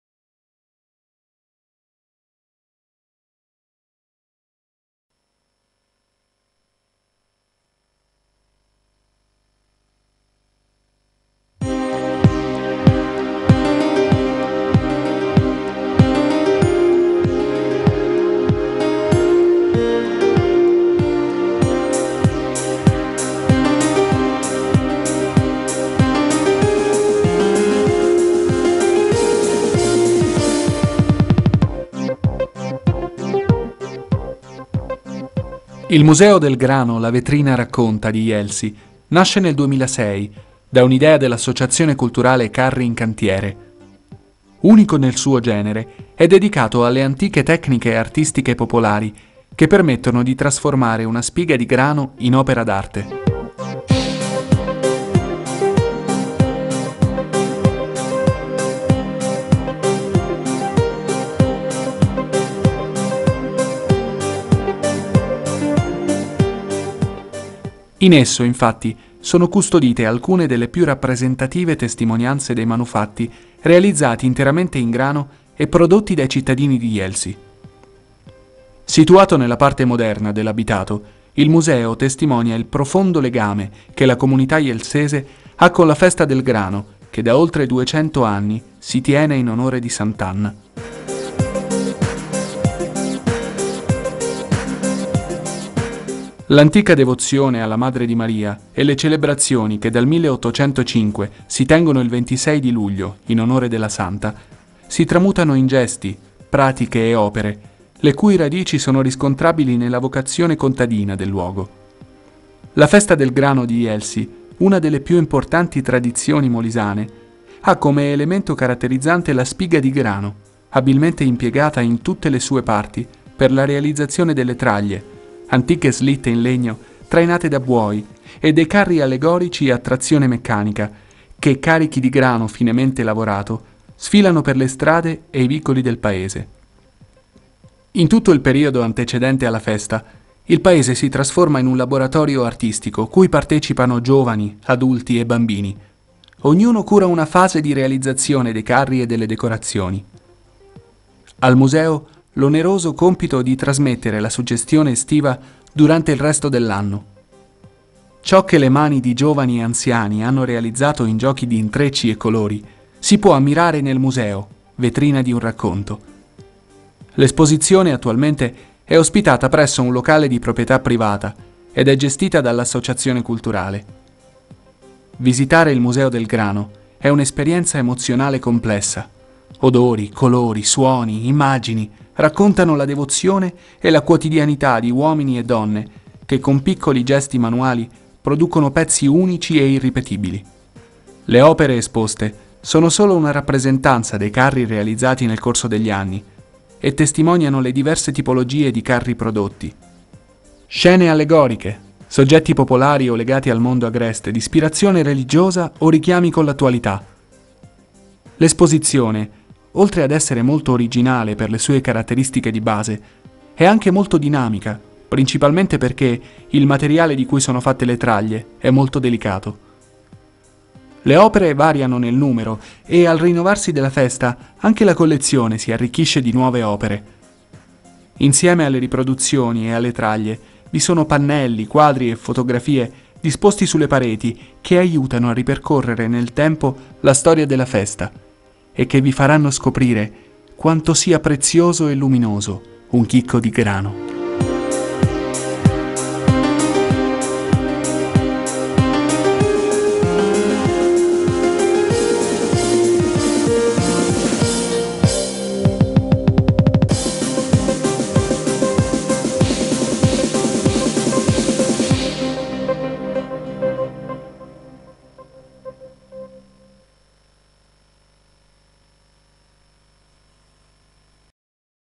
Jelsi, Museo del Grano
Il museo del grano è stato ideato e realizzato nel 2006 dall'Associazione culturale carri in cantiere che ancora oggi ne cura gli aspetti logistici ed organizzativi.  In breve tempo ha assunto rapidamente un ruolo di rilievo nel panorama storico-artistico regionale. Il contenuto del museo si rinnova continuamente: le opere esposte sono la sintesi più bella dei carri che vengono ogni anno realizzati in occasione della festività di S.Anna. All'interno è possibile, inoltre, scorgere fotografie E' aperto tutto l'anno, previa prenotazione telefonica o attraverso il sito web